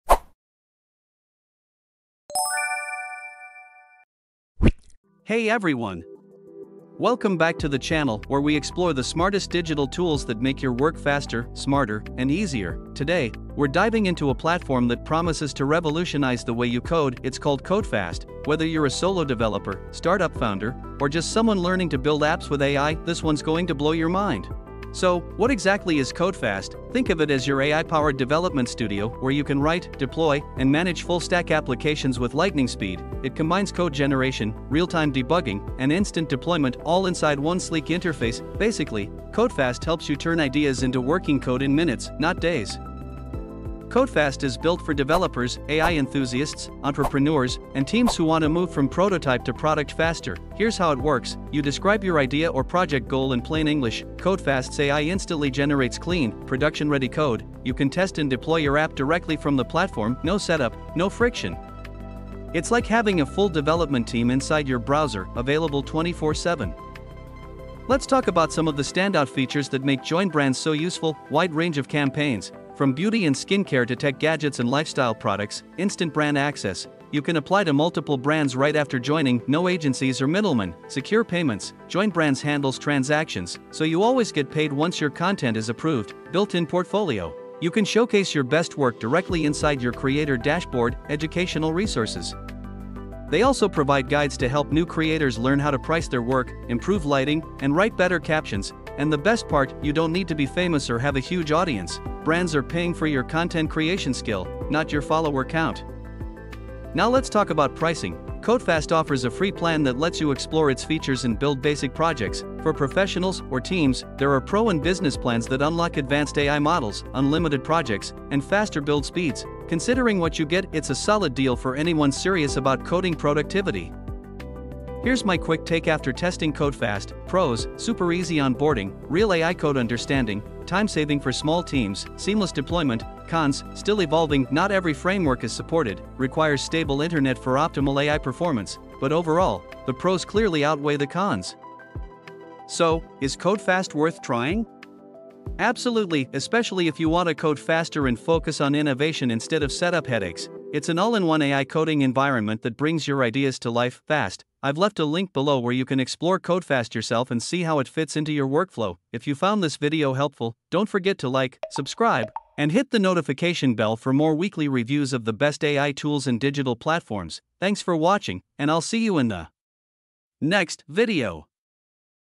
CodeFast Review 2025 – The Ultimate AI Coding Platform for Developers | DigitalBizSuite
Discover CodeFast – the next-generation AI coding platform that helps developers build, test, and deploy projects in minutes.

In this CodeFast review, I’ll show you how this platform transforms coding into a faster, smarter, and more creative process.

Whether you’re building your next SaaS product, experimenting with AI apps, or learning to code — CodeFast is your new secret weapon.

Check out CodeFast today and see how it can boost your workflow